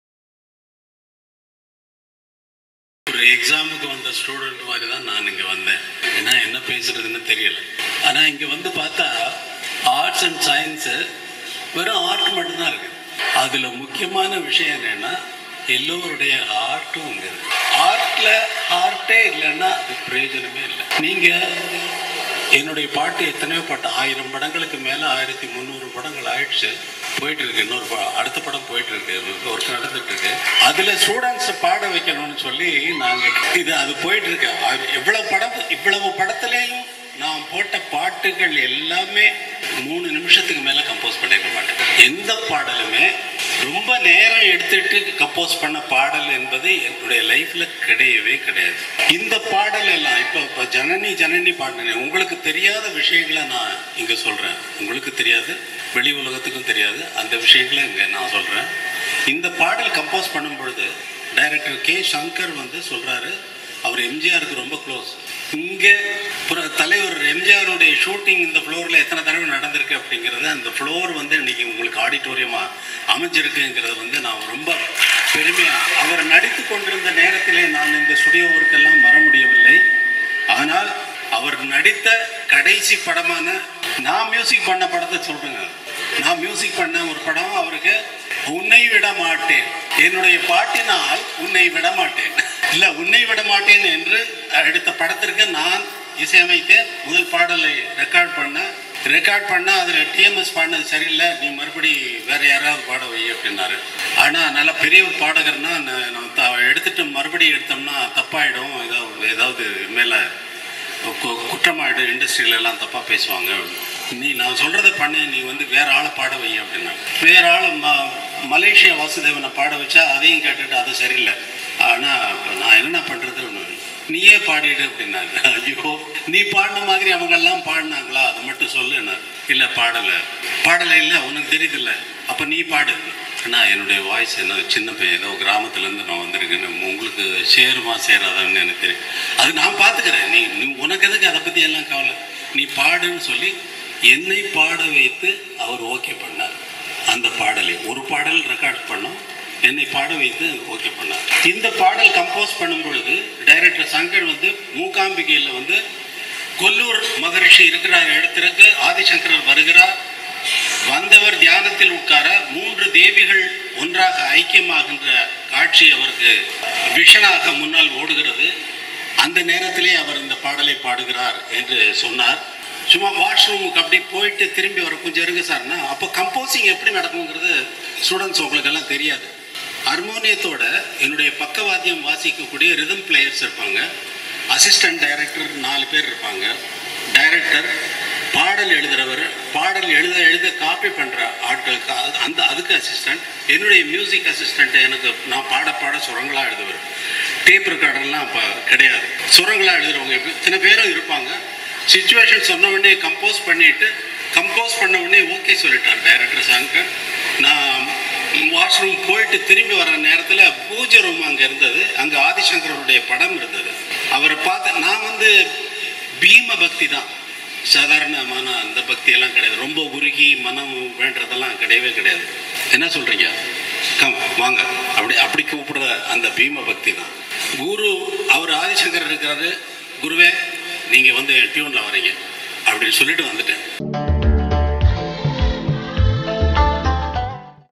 Ilayaraja 75th Birthday celebration @ MGR Janaki College
saignai Ilayaraja 75th Birthday celebration with MGR Janaki College Students